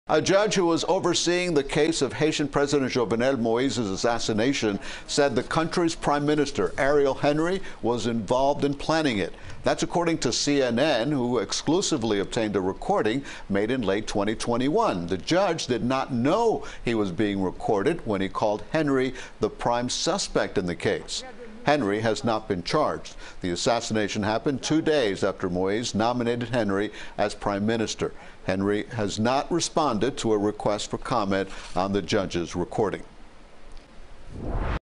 Judge: Haitian Prime Minister Involved In Planning President Jovenel Moïse's Assassination
In a recording obtained by CNN, a judge who was overseeing the case of President Jovenel Moise's assassination said Haitian Prime Minister Ariel Henry was involved in planning it.